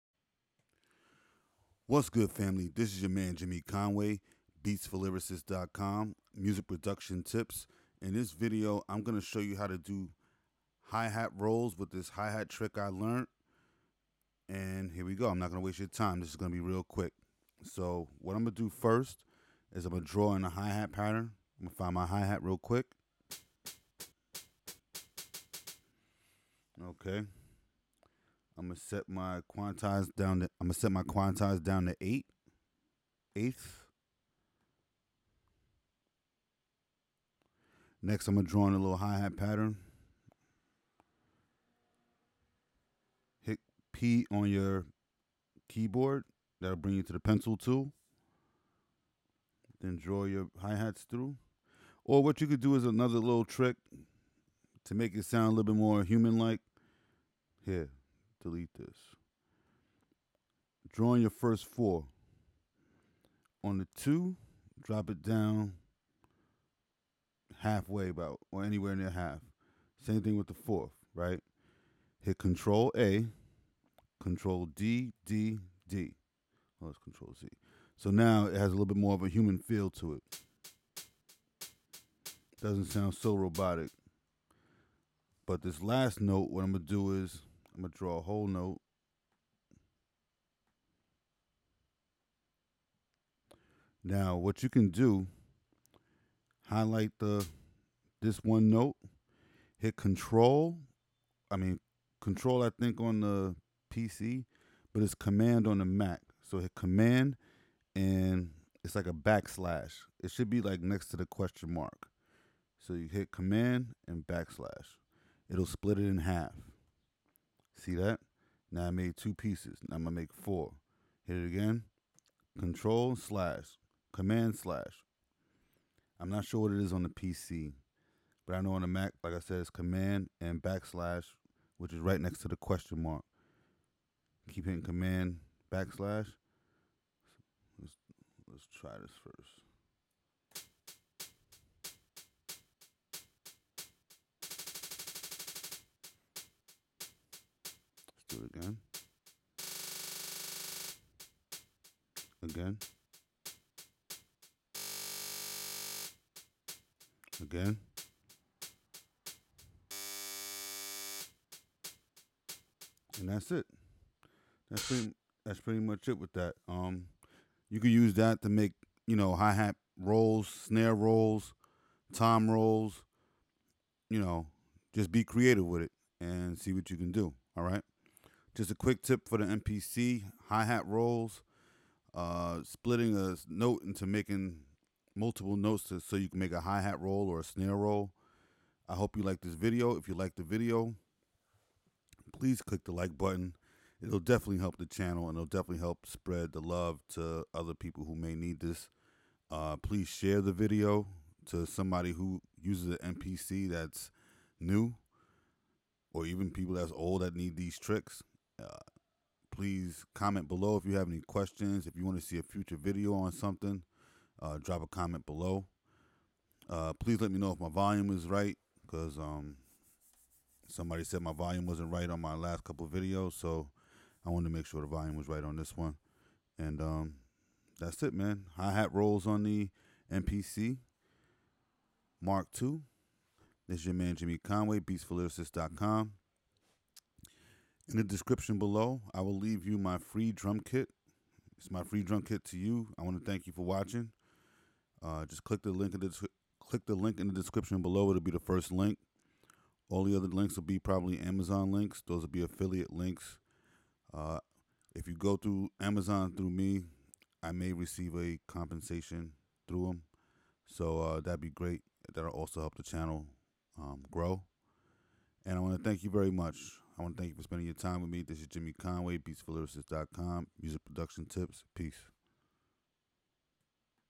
MPC Software Tutorials For Beginners - Hi Hat Rolls Made Easy
Macbook Pro
Audio Interface
Akai MPK MINI 3
MPC Studio MK2
Hello family.  Welcome to my new video MPC Software Tutorials For Beginners - Hi Hat Rolls Made Easy.
In this video i show you how to make hi hat rolls quick and easy using a shortcut.
If you hit command + /  on any midi event, it will split the midid event in half.  Try this when you're making your next hit.